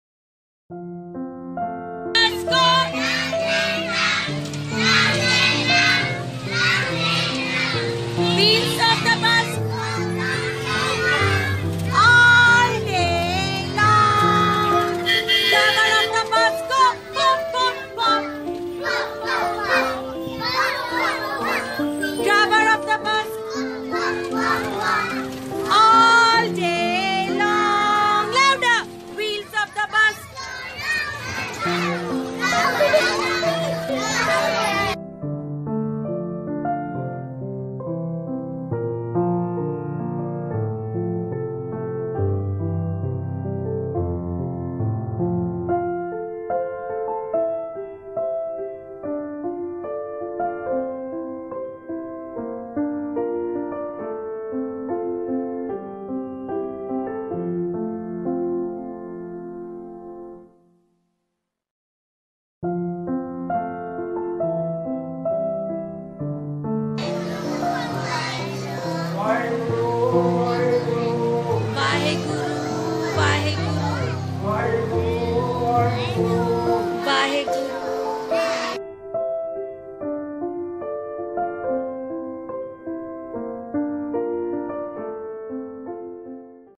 Visit to neighbouring places
Gurudwara visit by Nursery  kids